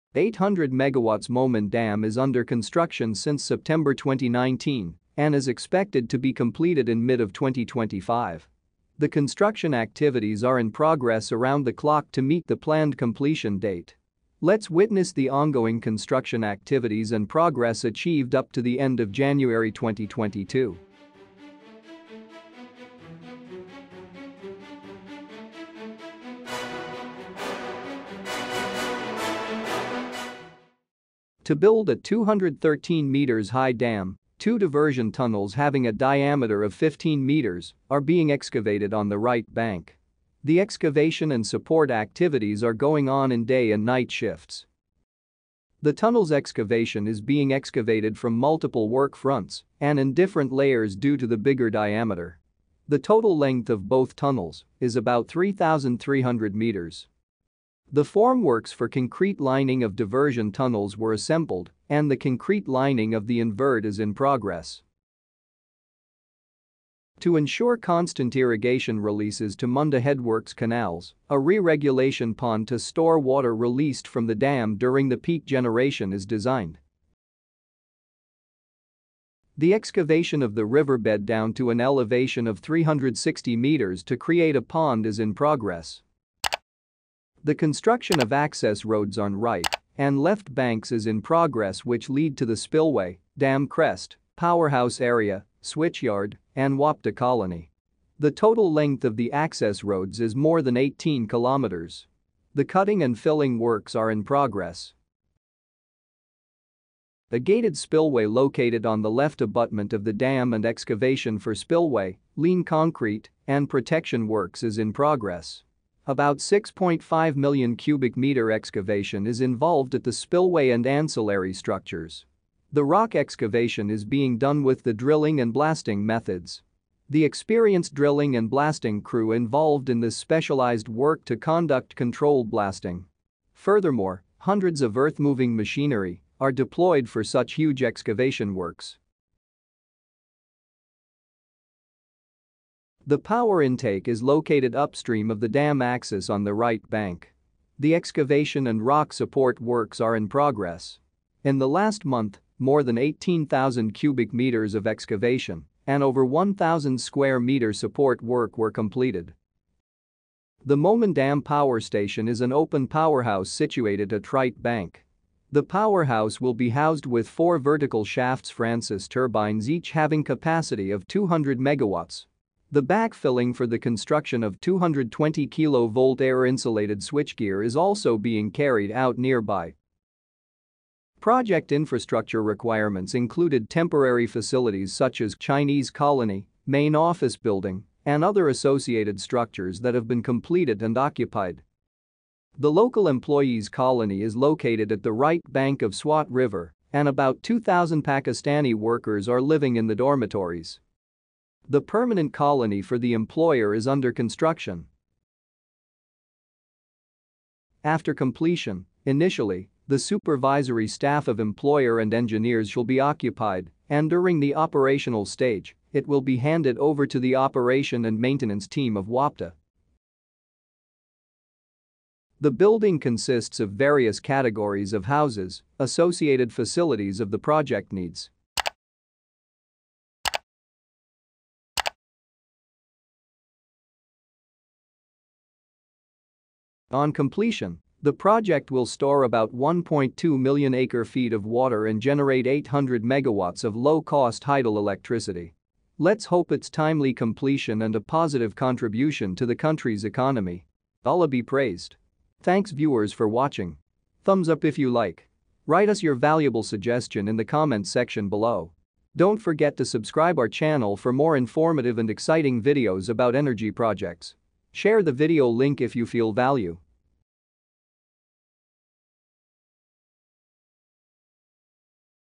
Mohmand Dam | Construction Progress | January 2022
800MW Mohmand Dam is under construction since September, 2019 and expected to be completed in mid of 2025. The construction activities are in progress around the clock to meet the planned completion date. Let’s witness the ongoing construction activities and progress achieved up to end of January, 2022.

To build 213m high dam, two diversion tunnels having diameter 15m are being excavated on the right bank. The excavation and support activities are going on in day and night shifts. The tunnels excavation is being excavated from multiple work fronts and in different layers due to bigger diameter. The total length of both tunnels is about 3,300m.
The formworks for concrete lining of Diversion tunnels were assembled and concrete lining of invert is in progress.

The construction of Access Roads on right and left banks is in progress which lead to spillway, dam crest, powerhouse area, switchyard and WAPDA colony. The total length of the access roads is more than 18 Kilometers. The cutting and filling works are in progress.
To ensure constant irrigation releases to Munda Headworks canals, a Re-regulation Pond to store water released from the dam during the peak generation is designed. The excavation of river bed down to elevation of 360m to create a pond is in progress.

A gated spillway located on the left abutment of the dam and excavation for spillway, lean concrete and protection works are in progress. At spillway and ancillary structures, about 6.5 million cubic meter excavation is involved. The rock excavation is being done with the drilling and blasting methods. The experienced drilling and blasting crew involved in this specialized work to conduct control blasting. Furthermore, hundreds of Earthmoving machineries are deployed for such huge excavation works.

The Power intake is located upstream of the dam axis on the right bank. The excavation and rock support works are in progress. In the last month, more than 18,000 cubic meters of excavation and over 1000 square meter support work were completed.

The Mohmand Dam power station is an open powerhouse situated at Right Bank. The powerhouse will be housed with four vertical shafts Francis turbines each having capacity of 200 megawatts. The backfilling for the construction of 220-kilo volt Air Insulated Switchgear is also being carried out nearby.

Project infrastructure requirements included temporary facilities such as Chinese Colony, main office building, and other associated structures that have been completed and occupied.

The local employees’ colony is located at the right bank of swat river and about 2000 Pakistani workers are living in the dormitories.
The permanent colony for the employer is under construction. After completion, initially, the Supervisory staff of Employer and Engineers shall be occupied and during operational stage, it will be handed over to Operation & Maintenance team of WAPDA. The building consists of various categories of houses, associated facilities of the Project needs.

On completion, the project will store about 1.2-million-acre feet of water and generate 800 megawatts of low-cost hydel electricity. Let’s hope it’s timely completion and positively contribution in the country’s economy.